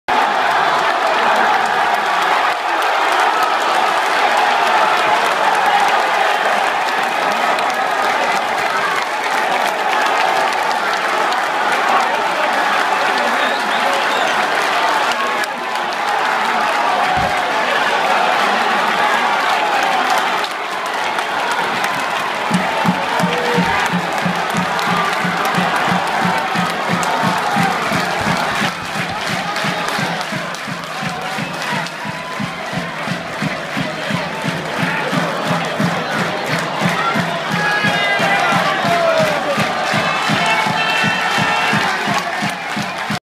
Castelbuonese - Sporting Taormina. L'esultanza per l'1 a 0.
Immagini esclusive bordo campo di Castelbuono.Org. L'esultanza dei giocatori e del presidente Fabio Capuana al 31° del primo tempo, in occasione dell'1-0.